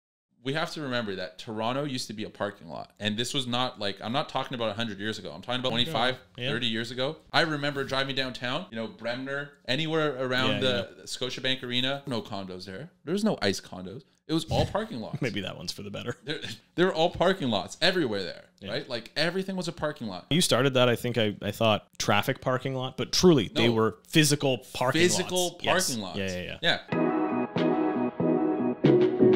ICE CONDOS BEFORE & AFTER
Toronto was developed into a suburban commuter heaven. Obviously that did not work out and things quickly began to change. Now all of the former wasted space (AKA parking lots) have been developed into mid/high rises, mixed use buildings, and entertainment complexes. Or in this case...trap houses 50 floors up in the sky lol